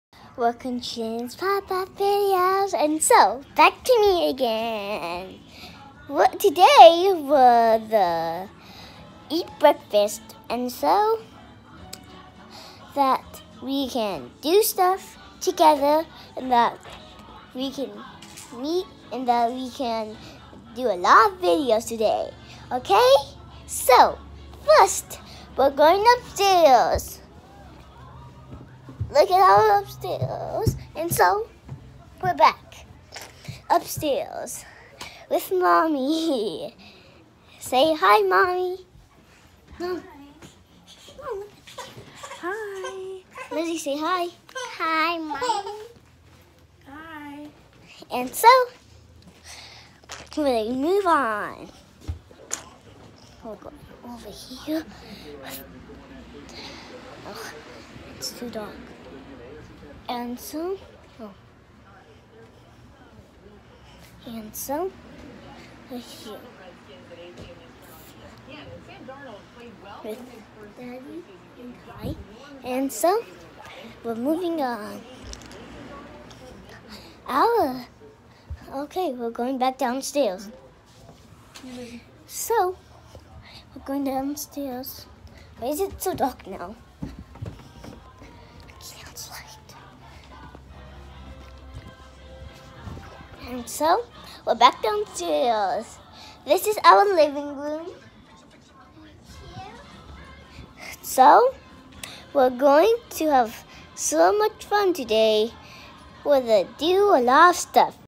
8/16/18 VLOG
A vlog for what our mornings look like during summer vacation